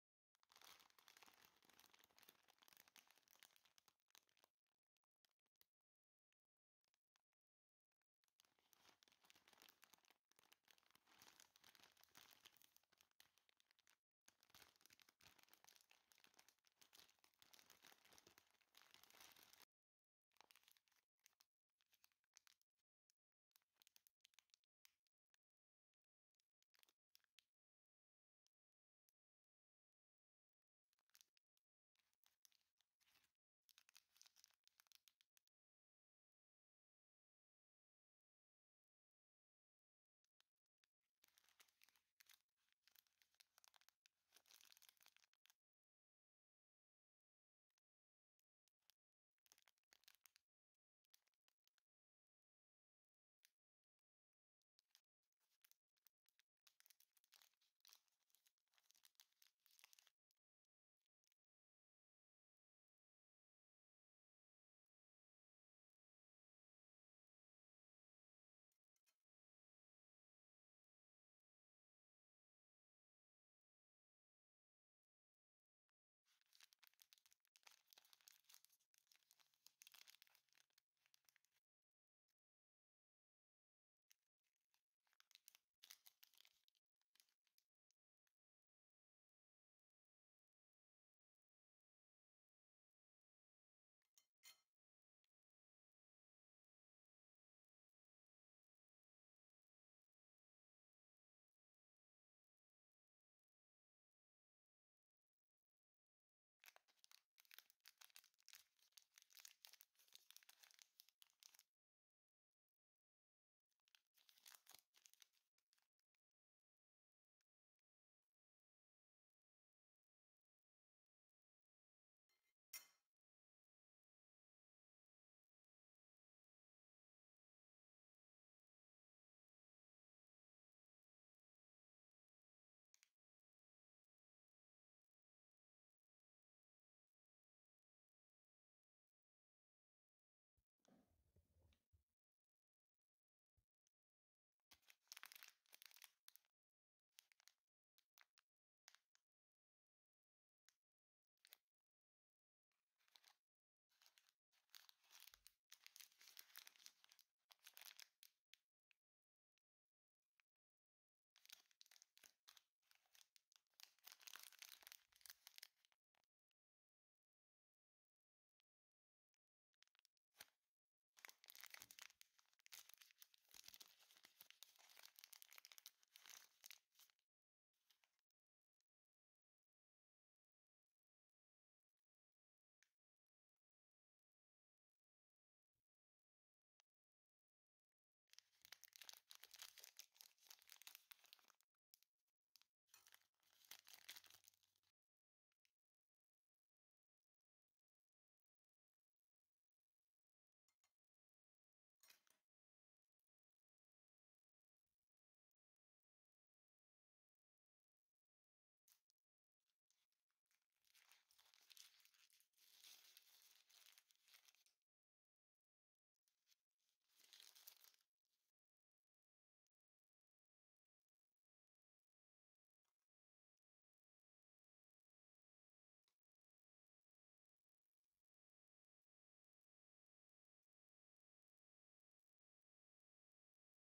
Trying Taiwan FAMOUS Fruity Mochi.🥭🍑
Trying 6 Exotic Mochi made in Taiwan. Mango, Lychee, Passion Fruit, Matcha, Milk and Red Bean.🇹🇼

1. Tropical Fruity Mochi pack (3 flavors)
2. Mixed Mochi pack (3 flavors)